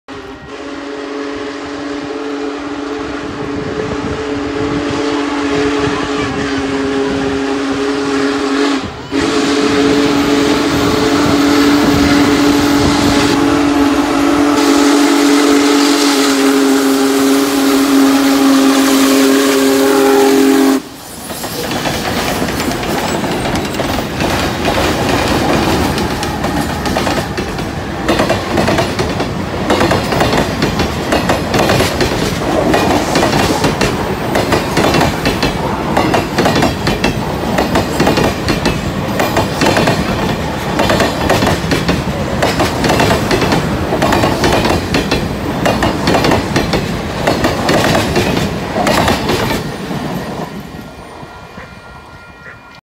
Union Pacific 4014 Big Boy - Crescent, IA 7/15/19
Union Pacific Big Boy traveling to Boone, IA on Monday 7/15.  Impressive sight to say the least.  Very fortunate to have the time to see it at work.